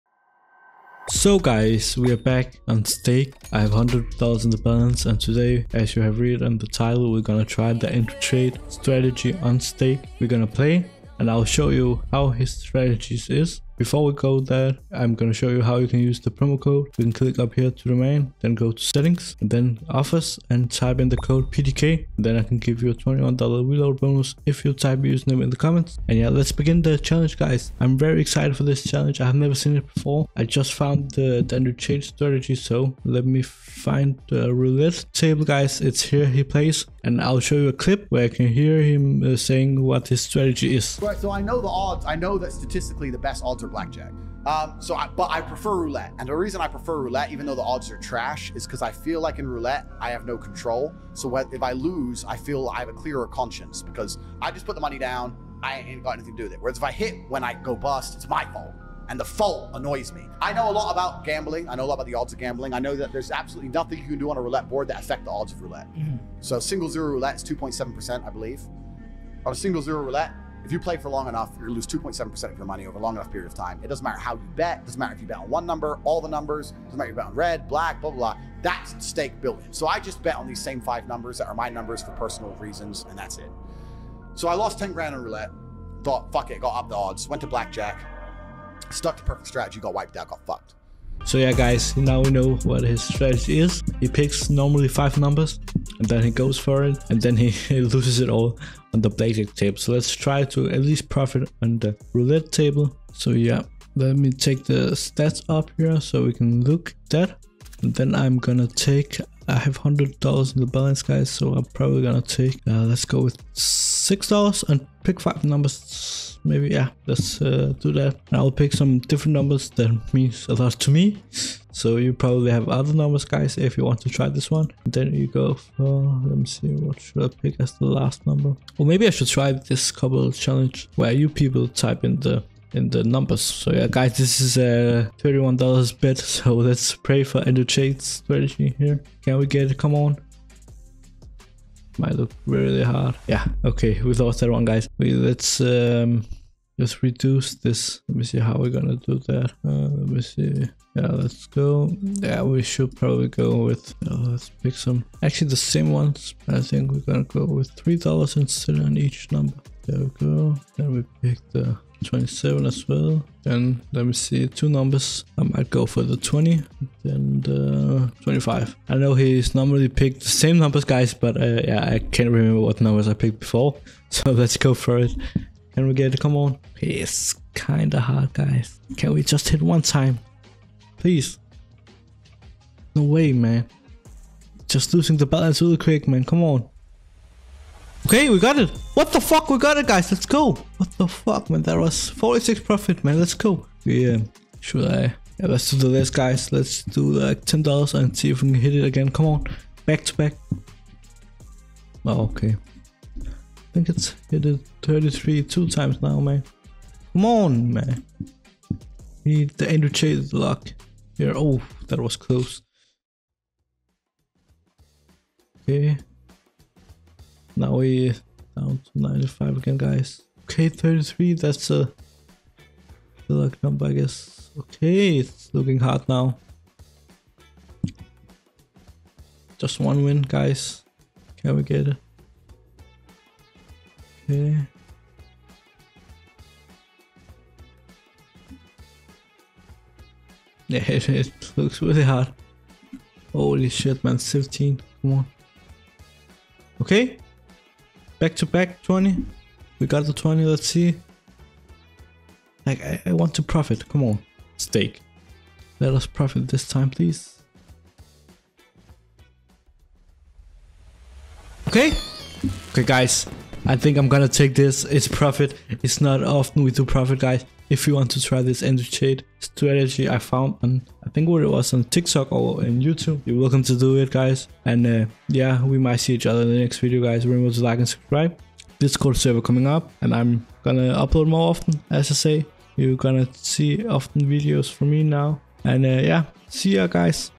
I Tried The Andrew Tate Roulette Strategy | Stake
Today, we're diving into Andrew Tate Roulette Strategy, be sure to watch the entire video.

⭐️ BONUS: register with code 'PDK' to claim $21 for FREE and unlock Cashback. 🎁